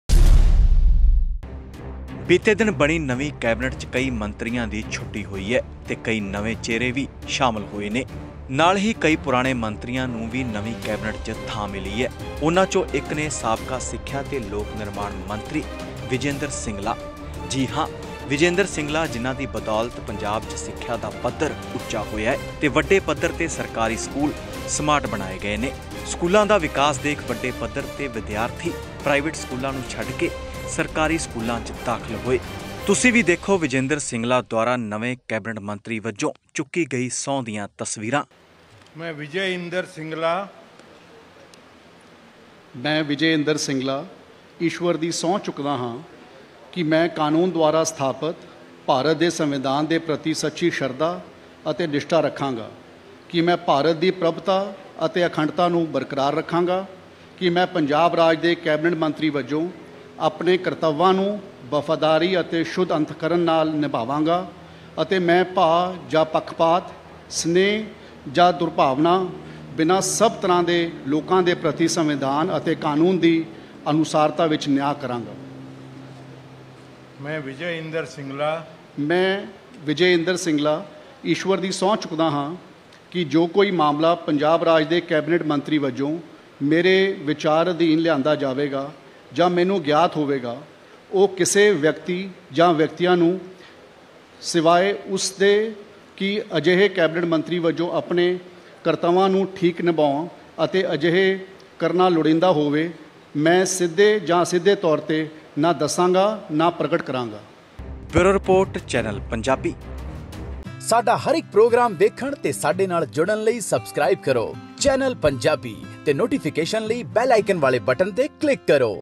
ਵਿਜੈ ਇੰਦਰ ਸਿੰਗਲਾ ਦੀ ਬੱਲੇ-ਬੱਲੇ,ਚੰਗੇ ਕੰਮਾਂ ਕਰਕੇ ਮੁੜ ਬਣਾਈ ਕੈਬਨਿਟ 'ਚ ਥਾਂ
Channel Punjabi Presents:
ਵਿਜੈ ਇੰਦਰ ਸਿੰਗਲਾ ਦੀ ਬੱਲੇ-ਬੱਲੇ
ਚੰਗੇ ਕੰਮਾਂ ਕਰਕੇ ਮੁੜ ਬਣਾਈ ਕੈਬਨਿਟ 'ਚ ਥਾਂ